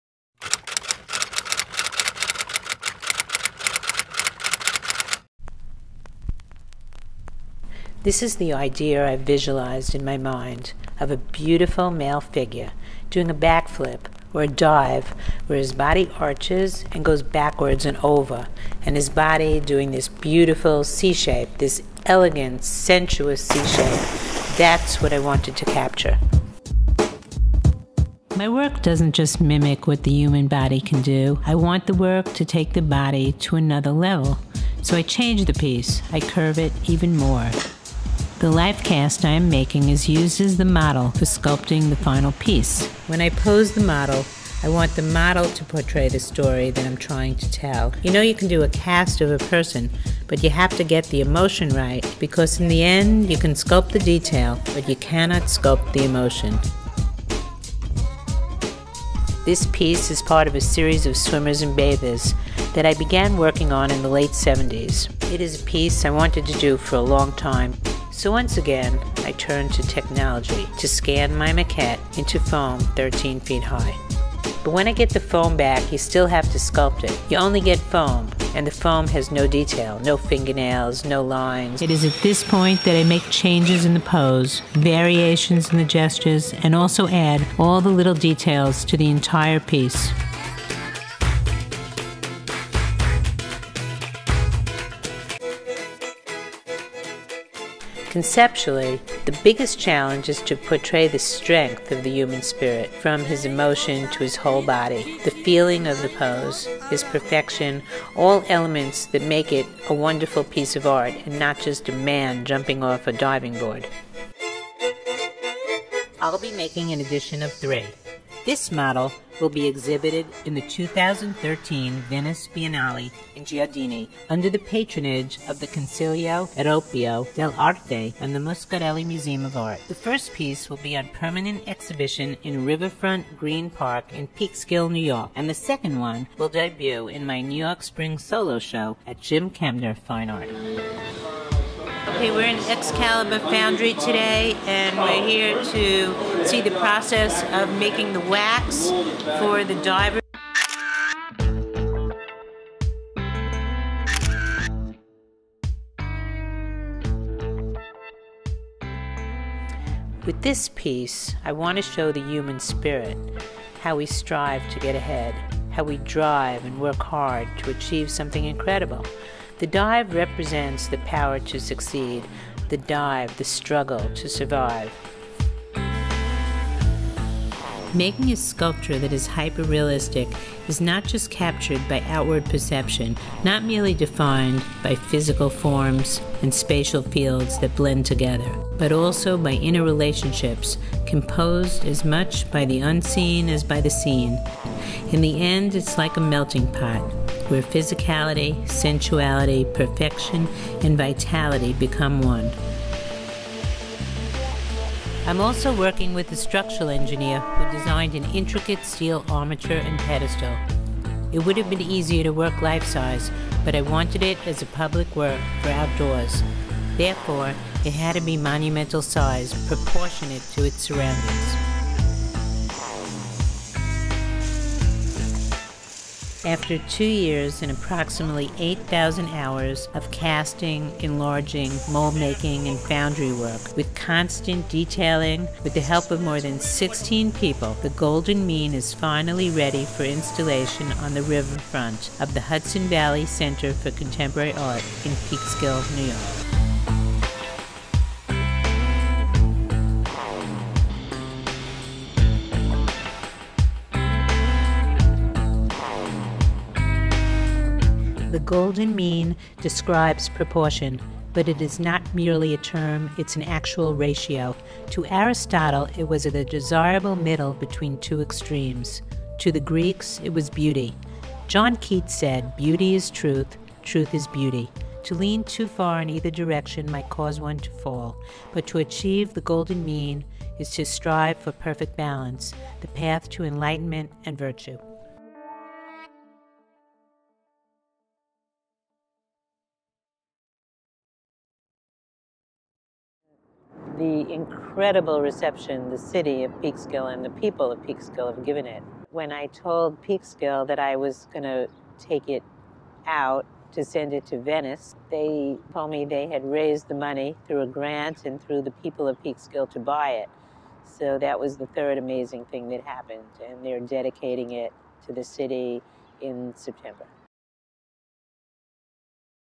"The Golden Mean”: A Masterpiece of Balance and Beauty by Carole A. Feuerman
Discover the Artistry: A Day in Carole A. Feuerman's Studio

Immerse yourself in the captivating world of Carole A. Feuerman's studio as she brings to life "The Golden Mean," a magnificent addition to her iconic series of swimmer sculptures. Standing tall at 16 feet and weighing nearly two tons, this bronze masterpiece depicts a male diver, gracefully balanced on his hands in an awe-inspiring arm stand position.

With the expert collaboration of a dedicated team of engineers, Feuerman defies the laws of physics, achieving a breathtaking equilibrium with a mere six inches of wrist. "The Golden Mean" serves as a powerful embodiment of the human spirit's resilience and strength, symbolizing life's intricate balancing act and the triumph of survival.

In both the process and final sculpture, Feuerman's artistic vision shines, transcending mere imitation and challenging perceived notions of reality. Witness her unwavering commitment to pushing artistic boundaries and capturing the essence of the human experience.

Captured through the lens of Alvaro Corzo V. (corzo360.com), this captivating video takes you behind the scenes, offering a glimpse into the extraordinary world of Feuerman's studio. Model Richard Nuzzolese lends his striking presence, adding depth to the artistic narrative.

Join us on this immersive journey, where artistry and imagination intertwine, showcasing the brilliance of Carole A. Feuerman's vision and her ability to inspire us all.